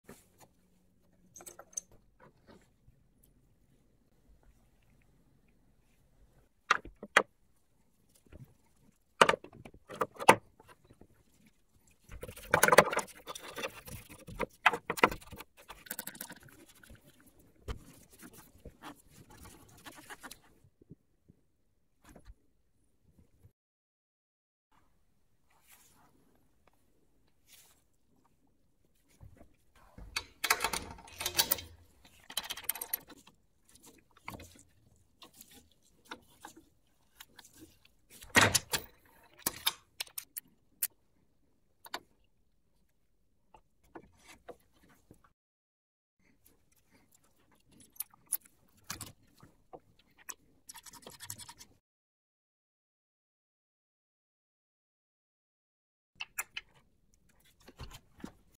First headset service since 2001. Old MTB build. Part 9 - headset bearings greasing and assembly.
- all kinds of headset bearings
- headset tools